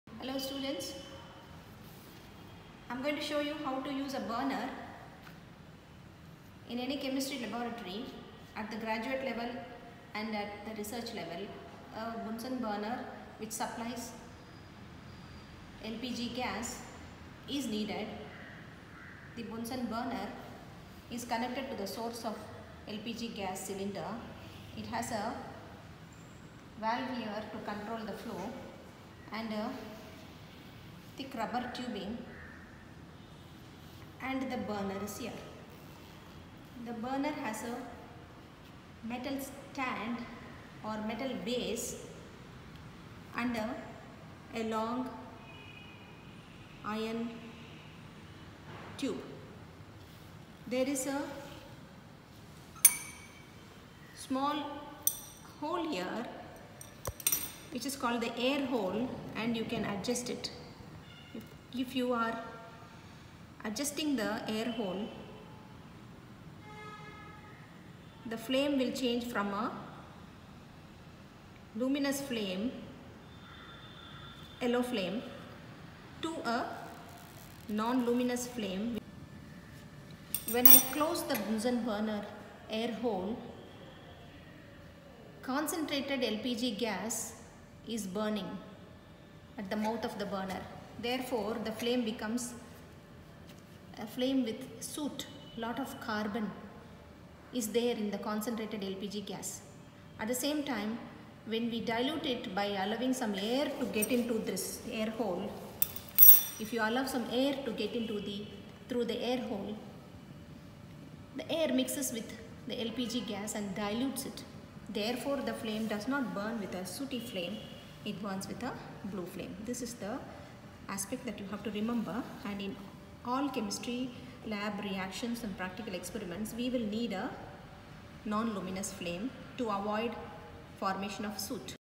Simple Lab Processes - How to use a Bunsen Burner in a Chemistry Lab
These videos are created for the reference of  graduate students and beginner researchers.